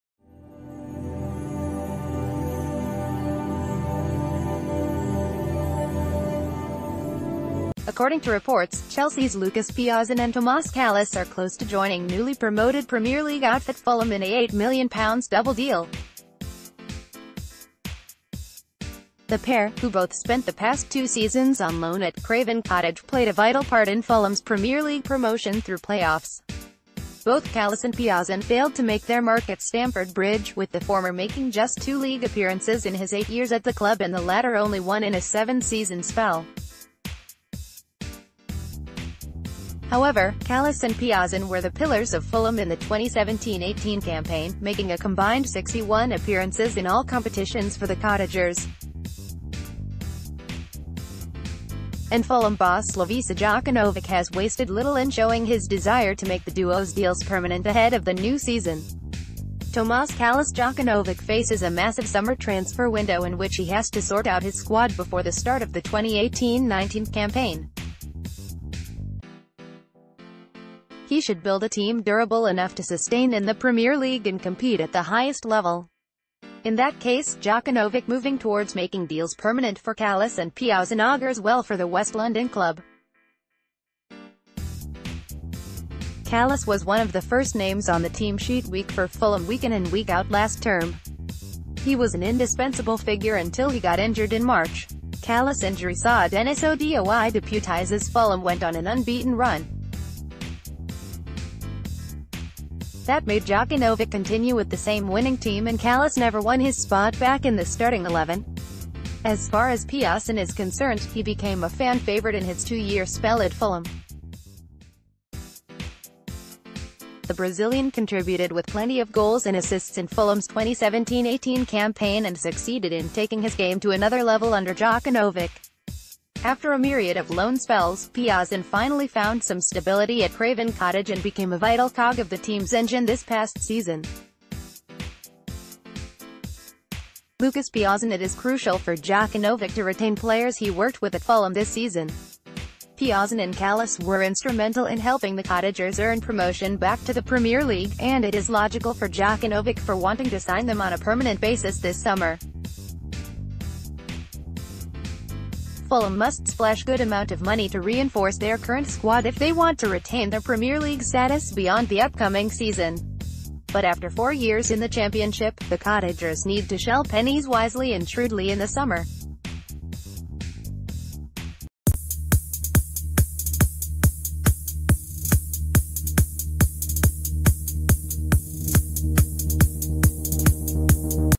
Breaking News | Jokanovic signing Chelsea duo Lucas Piazon and Tomas Kalas could be great for the c
Breaking News | Jokanovic signing Chelsea duo Lucas Piazon and Tomas Kalas could be great for the club

According to reports, Chelsea’s Lucas Piazon and Tomas Kalas are close to joining newly-promoted Premier League outfit Fulham in a £8m double deal. The pair, who both spent the past two seasons on loan at Craven Cottage, played a vital part in Fulham’s Premier League promotion through play-offs. Both Kalas and Piazon failed to make their mark at Stamford Bridge, with the former making just two league appearances in his eight years at the club and the latter only one in a seven-season spell. Howe...